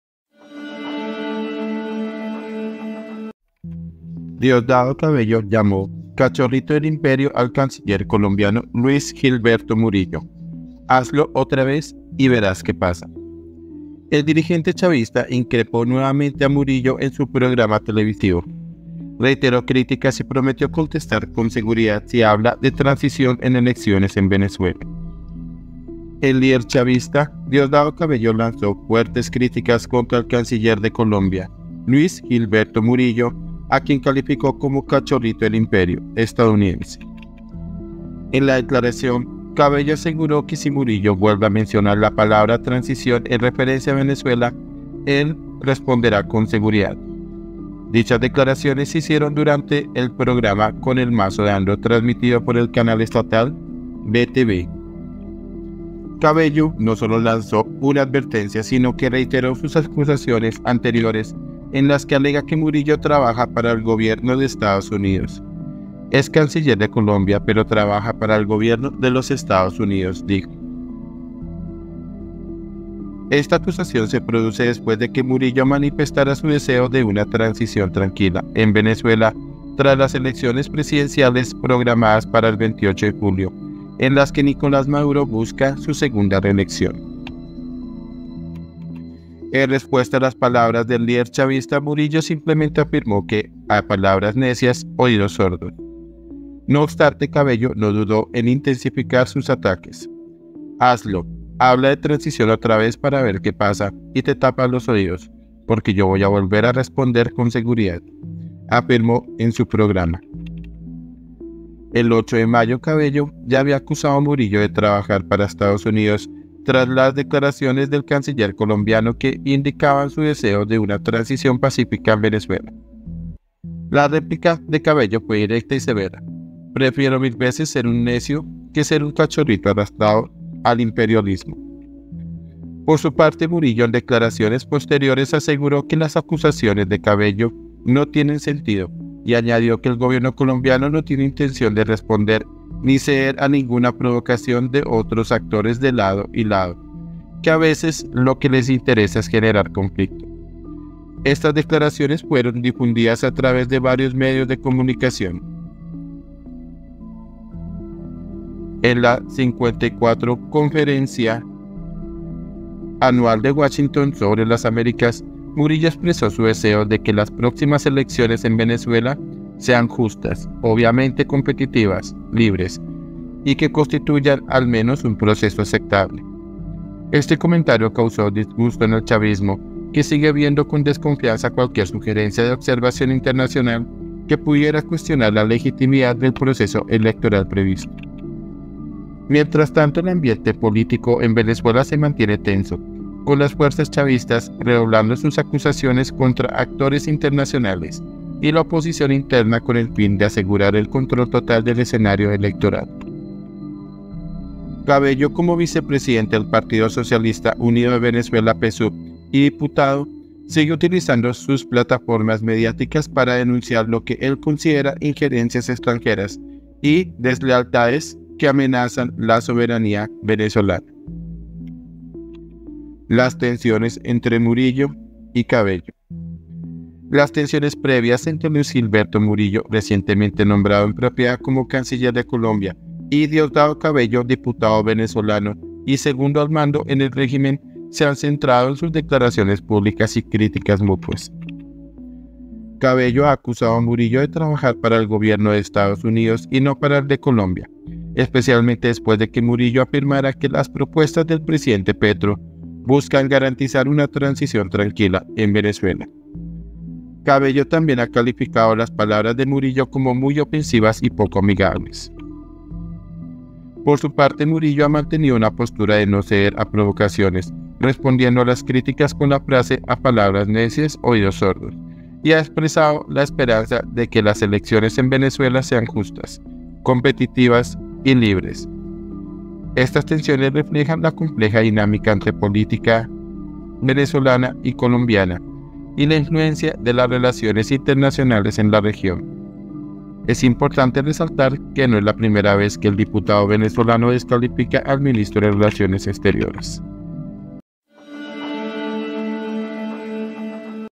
"CACHORRITO DEL IMPERIO" 🌐 DIOSDADO CABELLO ARREMETE CONTRA EL CANCILLER LUIS GILBERTO MURILLO
Diosdado Cabello llamó “cachorrito del imperio” al canciller colombiano Luis Gilberto Murillo: “Hazlo otra vez y verás qué pasa”
El dirigente chavista increpó nuevamente a Murillo en su programa televisivo. Reiteró críticas y prometió contestar “con seguridad” si habla de transición en elecciones venezolanas
INFOBAE